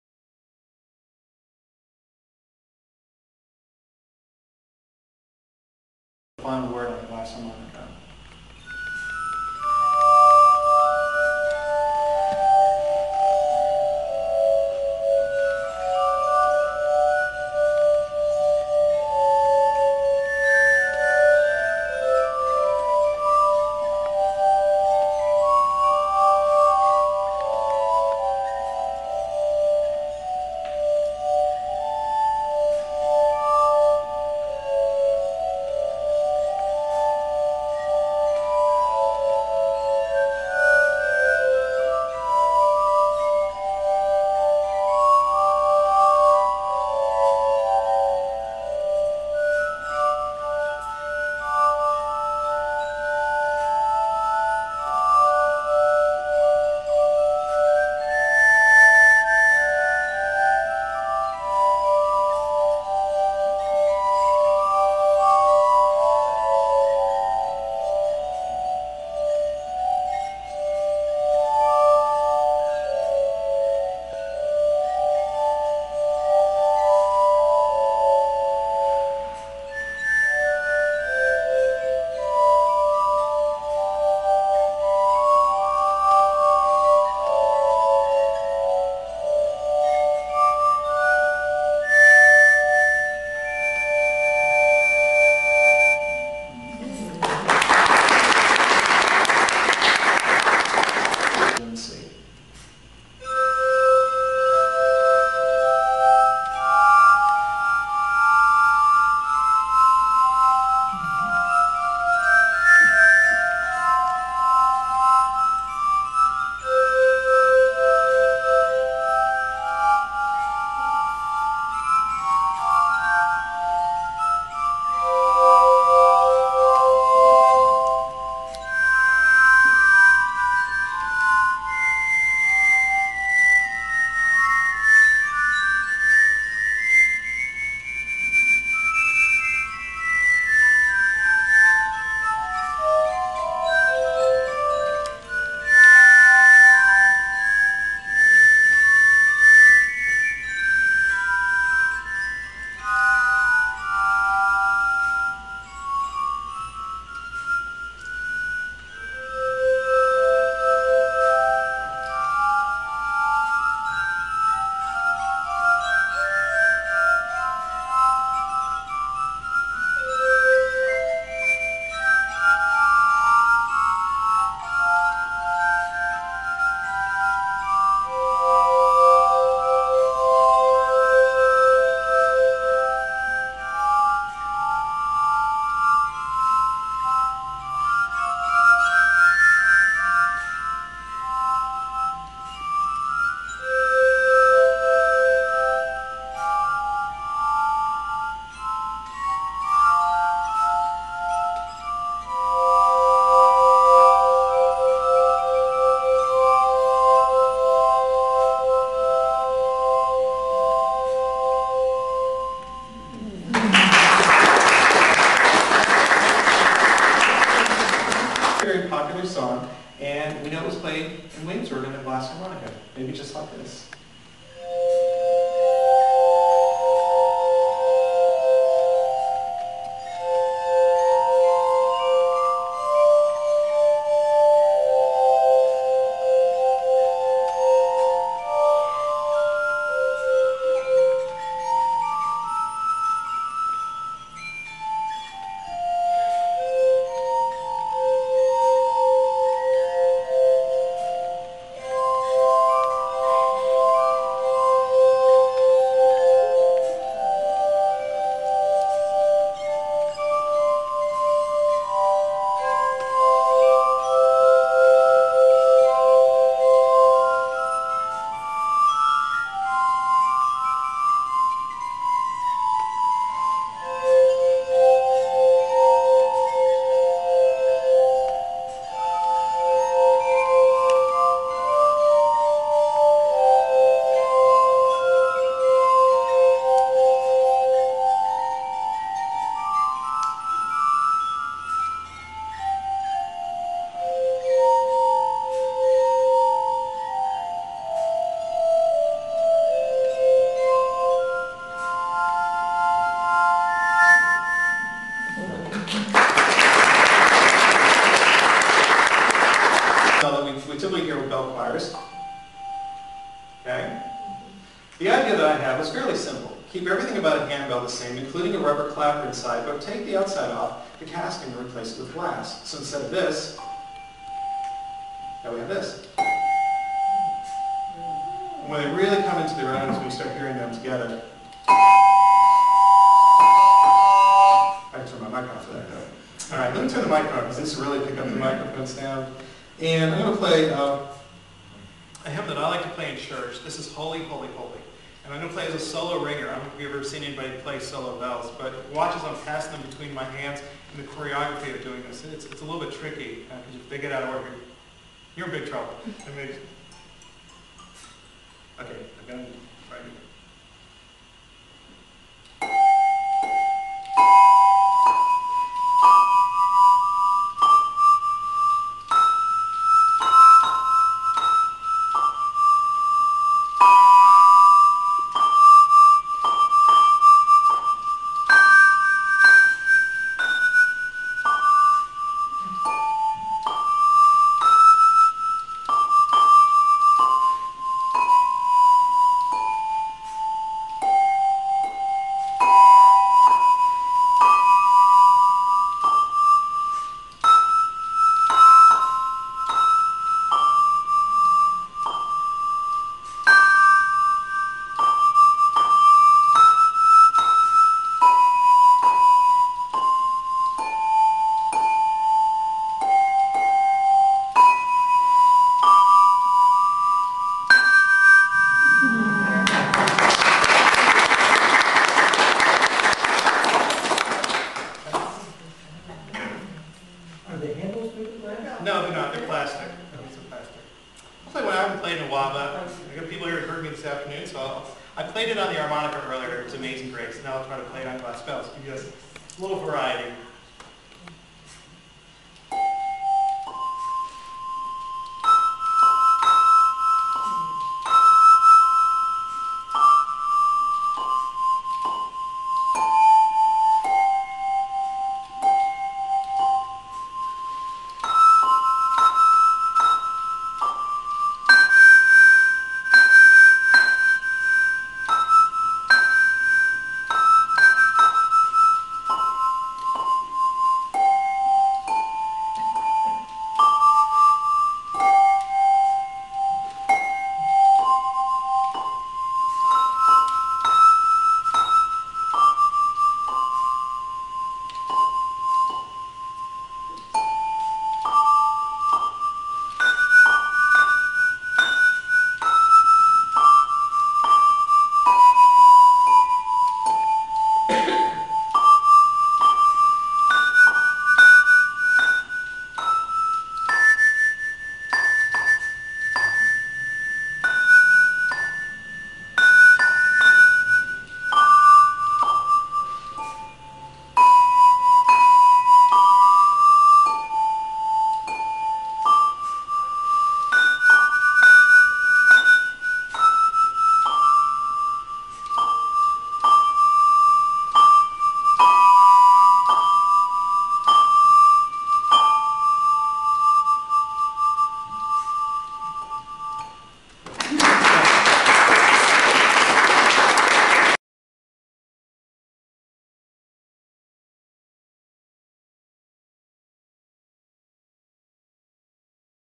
Crystal Concert At Glenwood Retirement Communities
Dean Shostak's Crystal Concert is a multi-media concert that introduces audiences to amazing glass instruments from Benjamin Franklin's Glass Armonica in 1761 to brand new musical instruments being created for Dean by glass artist all around the world.  The Crystal Concert is an ever changing showcase of glass music, art, history, and science.  The music ranges from Mozart and Beethoven, to modern day favorites.  Dean Shostak has been featured on "All Thing's Considered," "The Early Show," "Mr. Roger's Neighborhood," CNN, HGTV, and many other programs. They even use his music on The Weather Channel.